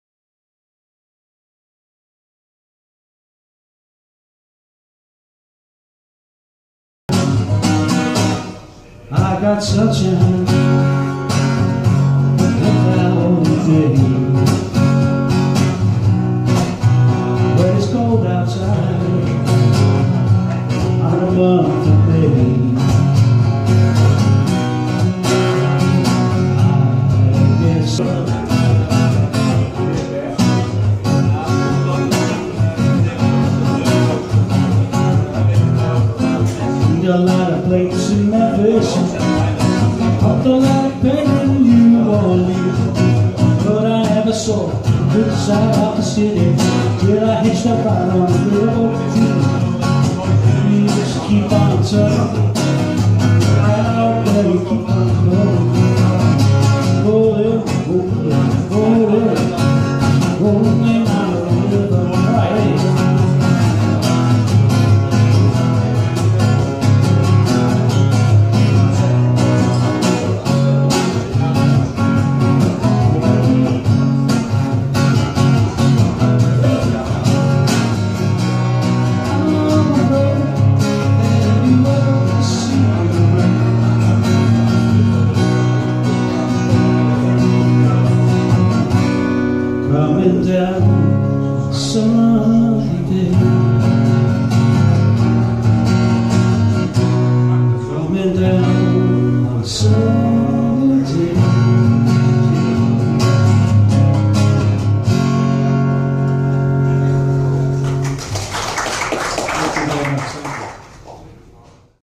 John McMinn - DFDS session October 2017
A brief video of me onstage at DFDS Newcastle/ Amsterdam